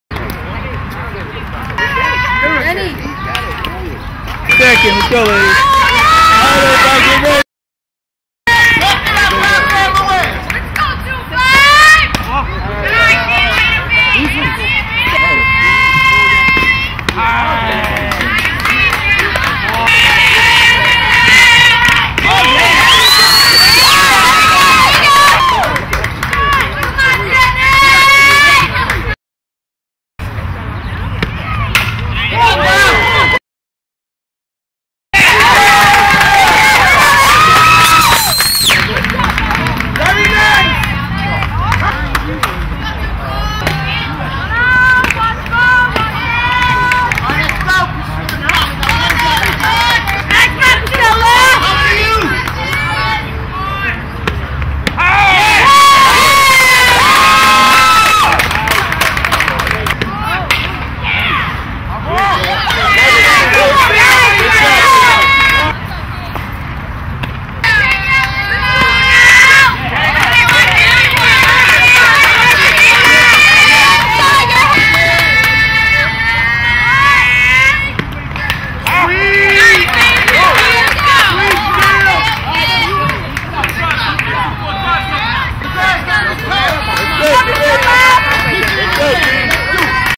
05.22.12 Madison vs. Francis Lewis Part 1
05.22.12 Madison vs. Francis Lewis -Francis Lewis eliminates Madison Lady Knights. Patriots advance into PSAL Varsity A softball semifinal.